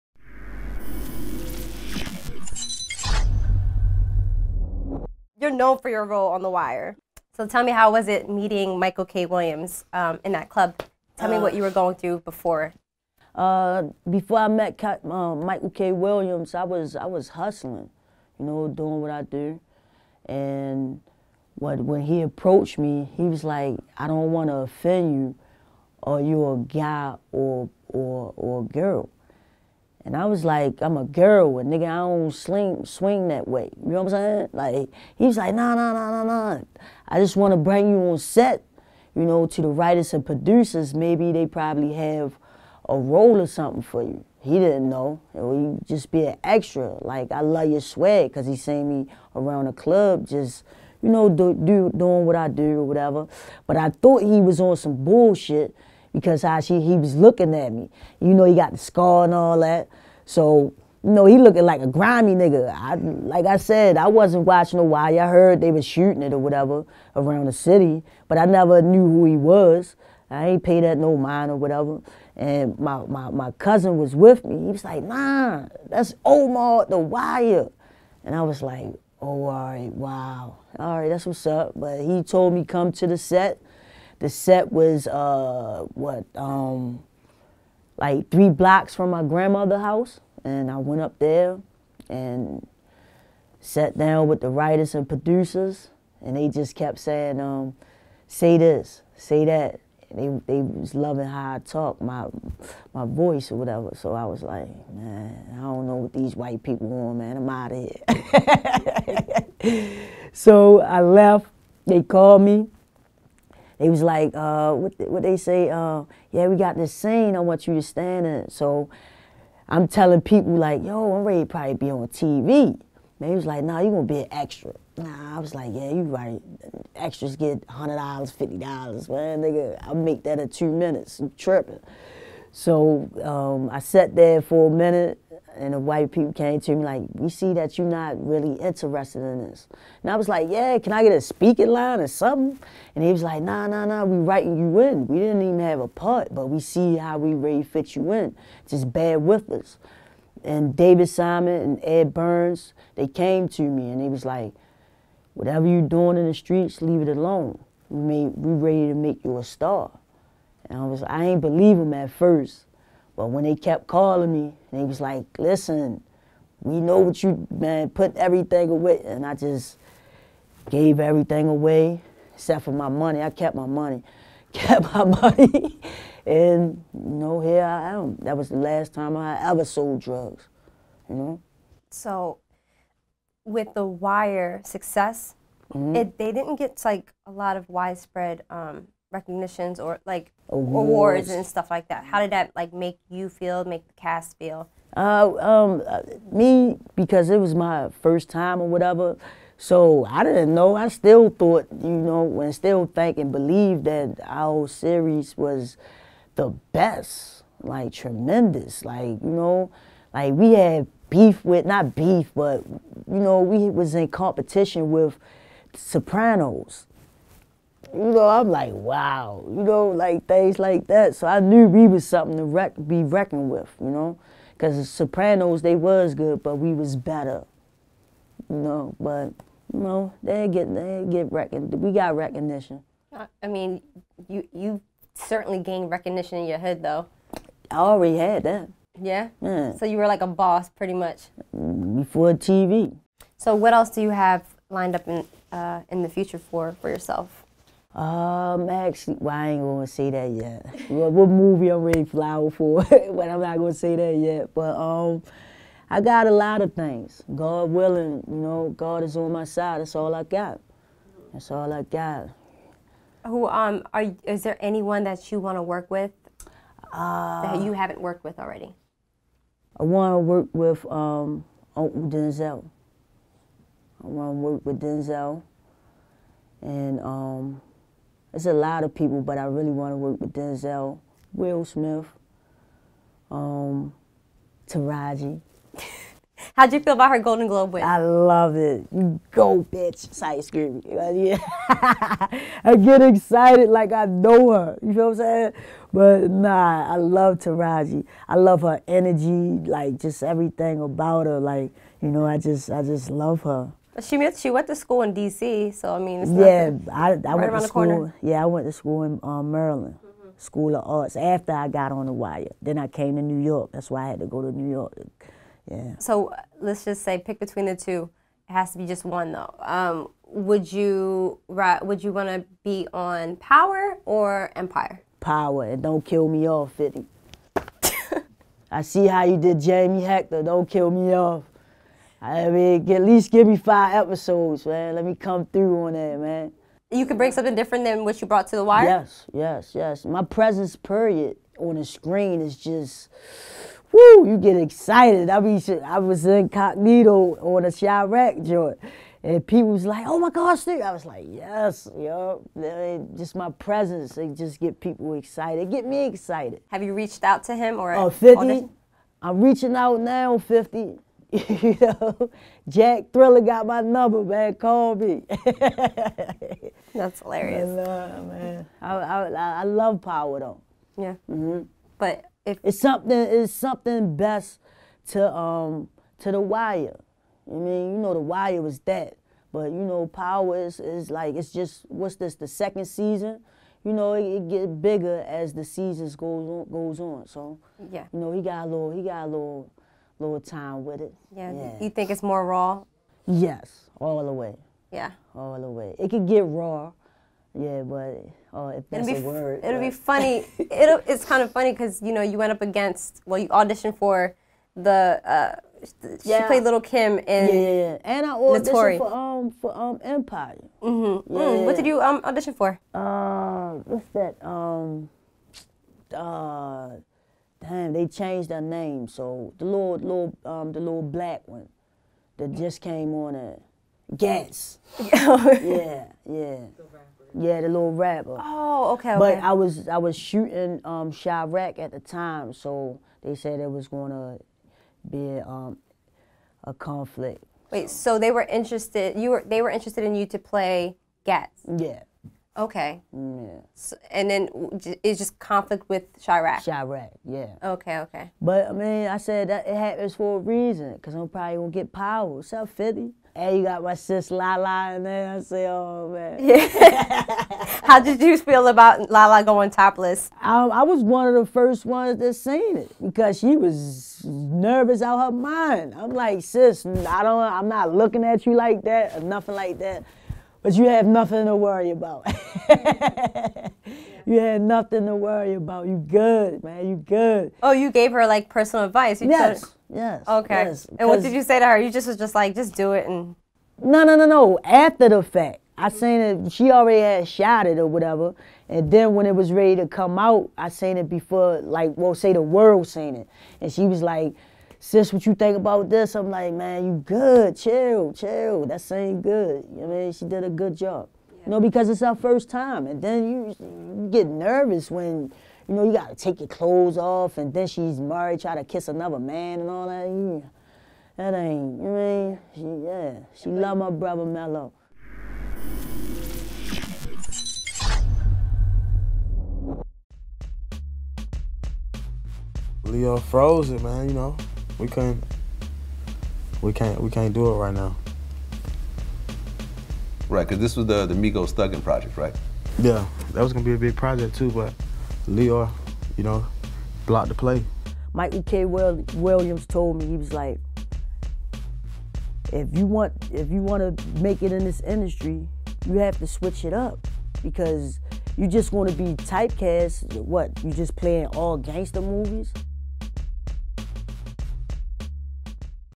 Felicia "Snoop" Pearson on Why She'd Rather Act in "Power" Over "Empire"
During our talk with Felicia "Snoop" Pearson of The Wire fame, the actress detailed how she was casted onto the show and which ones she's been eyeing to appear on next. When asked to choose between Empire and Power, Pearson picked the former despite being offered a role on FOX Network's hit show at one point. During the filming of Empire's season Felicia was set to appear as Freda Gatz (played by Bre-Z) after auditioning, but couldn't due to her shooting scenes for Spike Lee's Chiraq.

Felicia Pearson later goes on to explain why she'd like to be casted on the next season of Power. Her one plea to 50 Cent? "Don't kill me off," she says with a smile. "At least give me five episodes."

Press play for more.